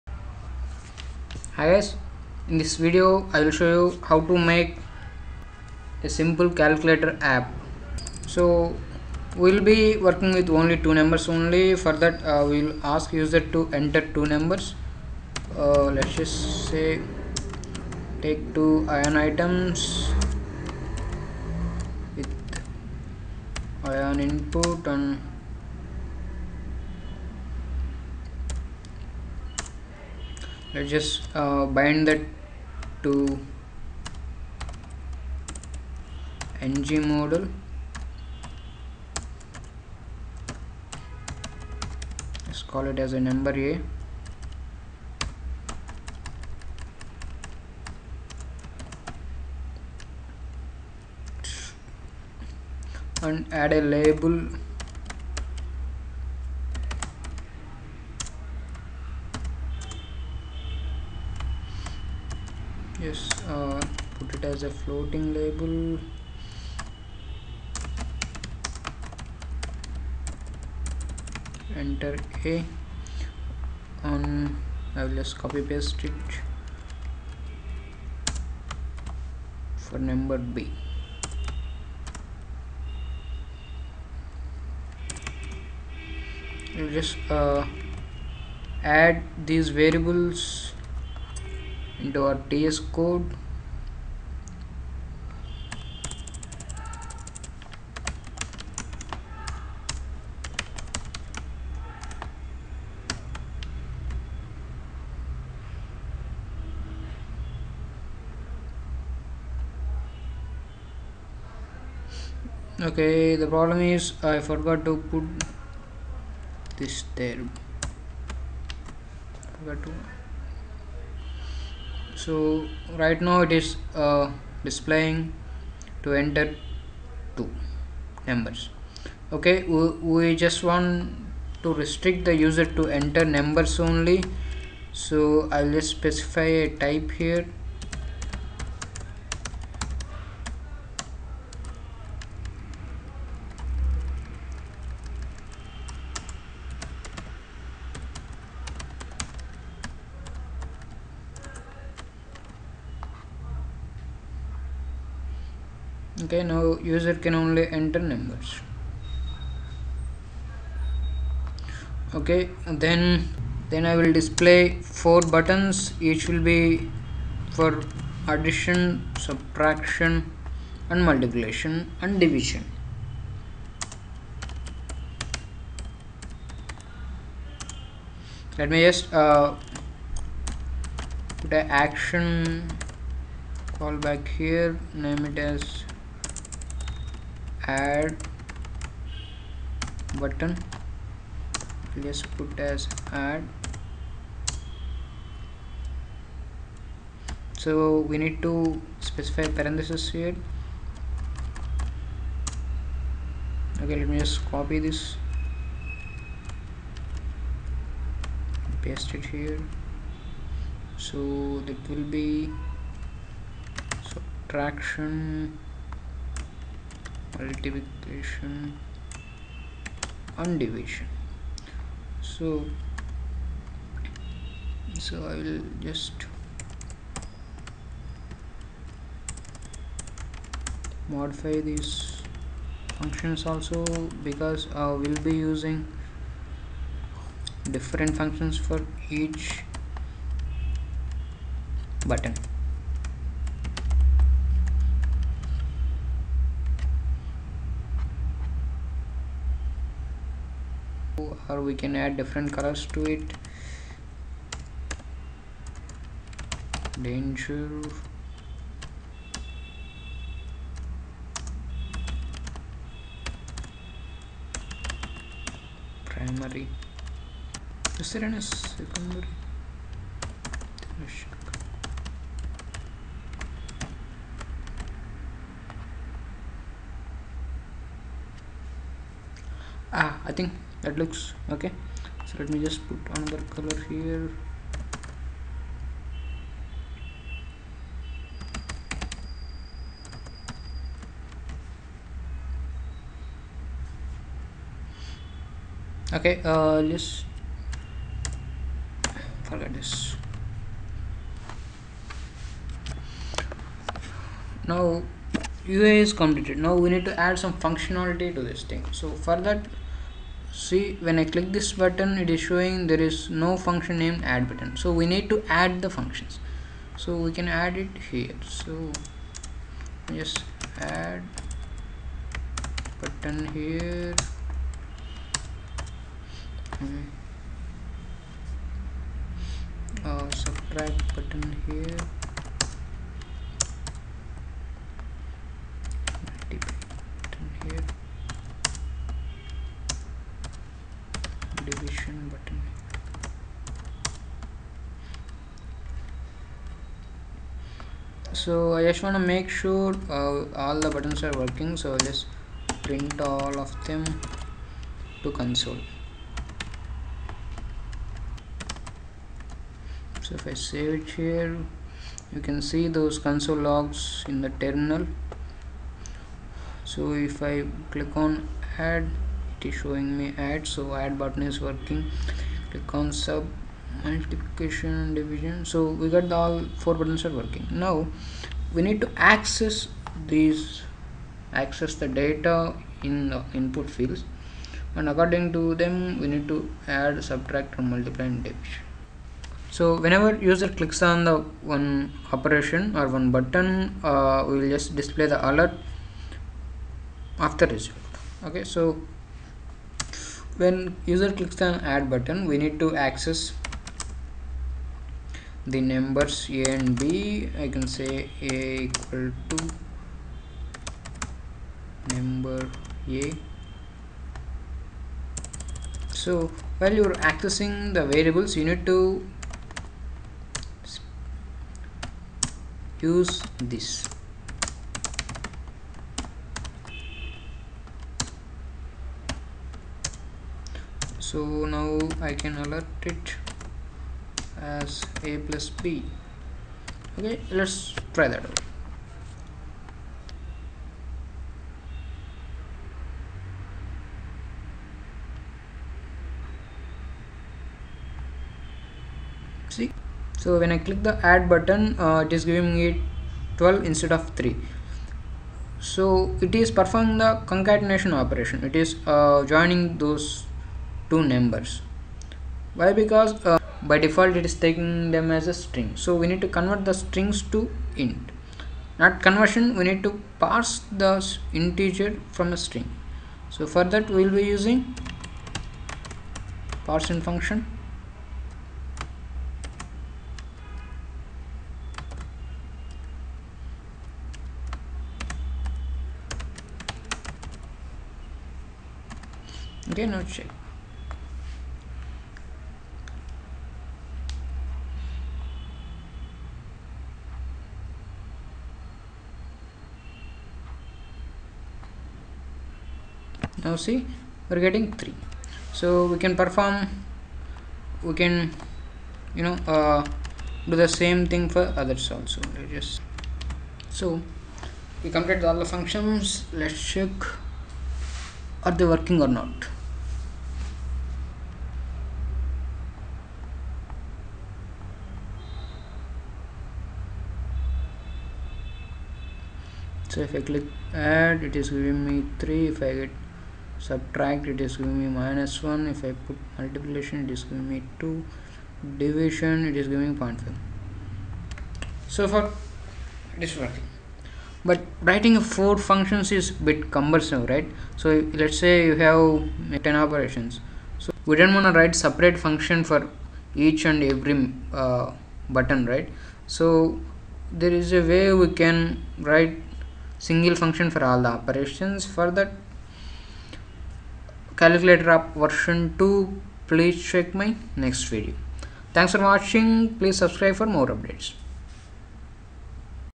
Ionic 3 for Beginners : Simple Calculator app V1 - along with source code
Are you struck on some problem ? looking for new team member on your exciting project. Do let me know I'm looking for freelancing opportunities.

In this Video i will show you how to make simple Ionic3 android  App